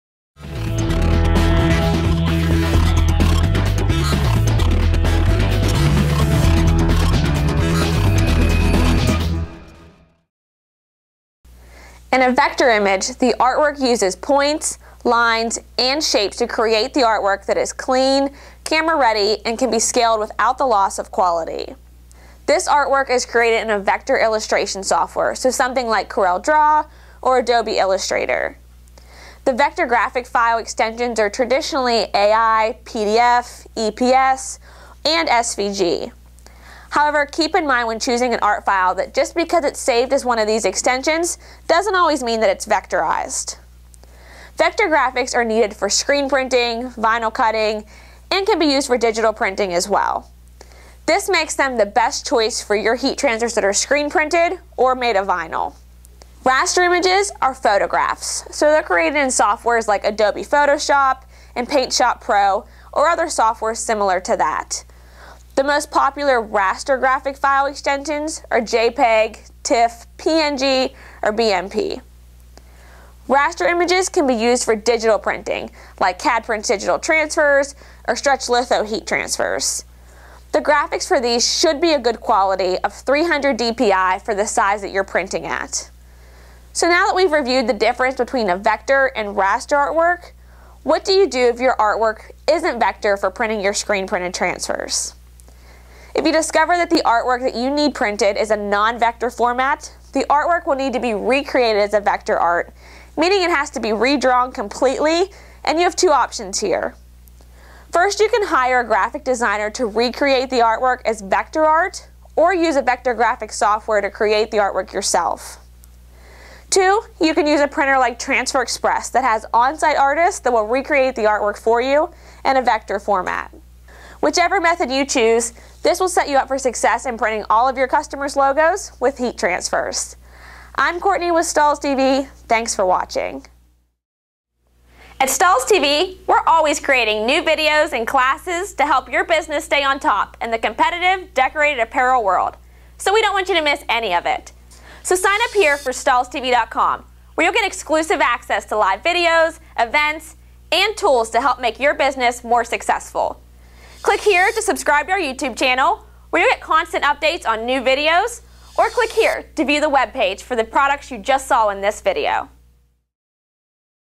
Best Artwork Files for Heat Transfers | Transfer Express
Artwork is the most important part of decorated apparel, but it can sometimes be confusing. If you ever wondered what the difference between raster and vector artwork was, or how you can create artwork suitable for heat printing, then watch this video with Stahls' TV educator Courtney Kubitza.